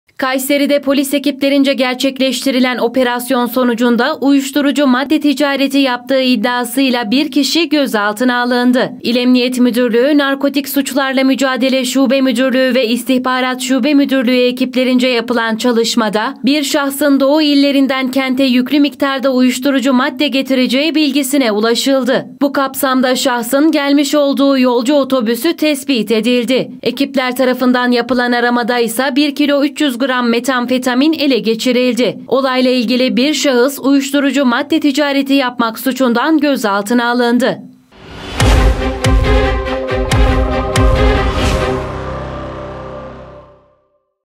Uyuşturucu Operasyonunda 1 Gözaltı
KAYSERİ (1HA) – Kayseri’de polis ekiplerince gerçekleştirilen operasyon sonucunda uyuşturucu madde ticareti yaptığı iddiasıyla 1 kişi gözaltına alındı.
İl Emniyet Müdürlüğü Narkotik Suçlarla Mücadele Şube Müdürlüğü ve İstihbarat Şube Müdürlüğü ekiplerince yapılan çalışmada, Ö.D. isimli şahsın Doğu illerinden kente yüklü miktarda uyuşturucu madde getireceği bilgisine ulaşıldı. Bu kapsamda şahsın gelmiş olduğu yolcu otobüsü tespit edildi. Ekipler tarafından yapılan aramada ise 1 kilo 300 gram metamfetamin ele geçirildi.
Olay ile ilgili Ö.D. isimli şahıs ‘uyuşturucu madde ticareti yapmak’ suçundan gözaltına alındı.